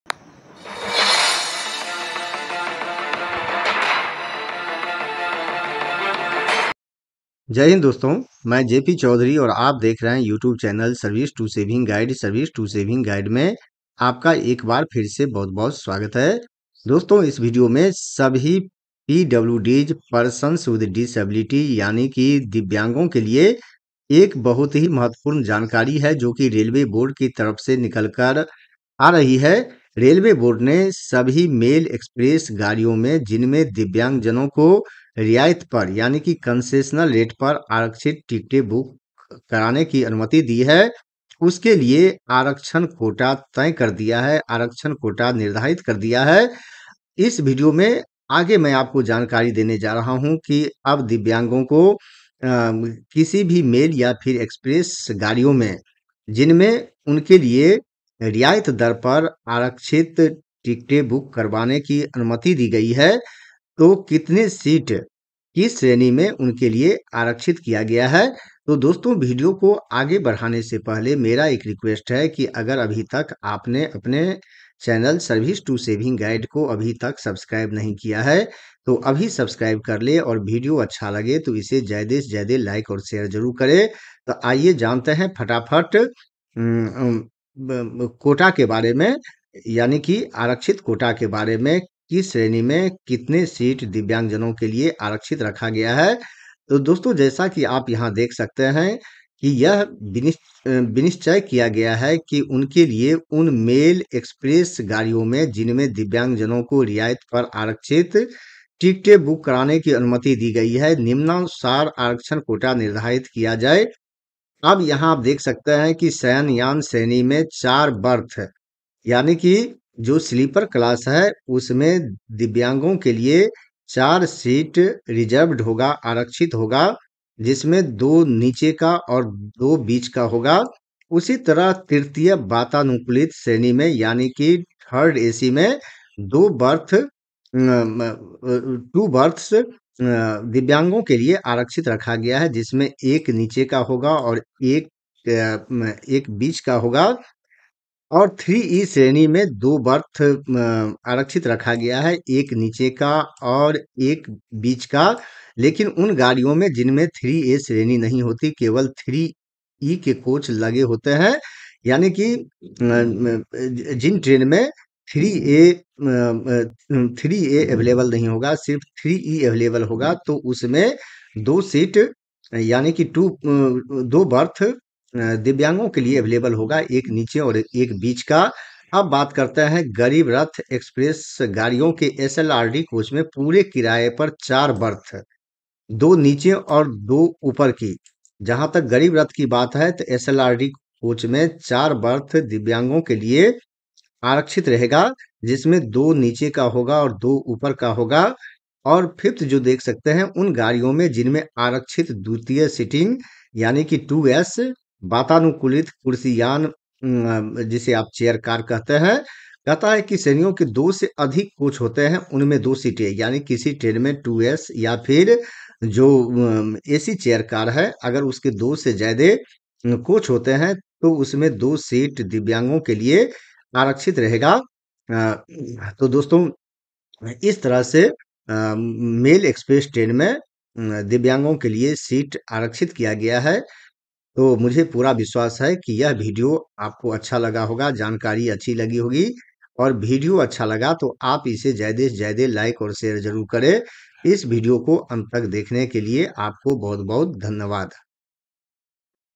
Reserved berths in trains for handicapped persons|अब इतने सीट दिव्यांगो के लिए ही आरक्षित रहेगा!
Railway board has reserved berths for handicapped persons in trains in Sleeper,AC3,2S and AC chair car.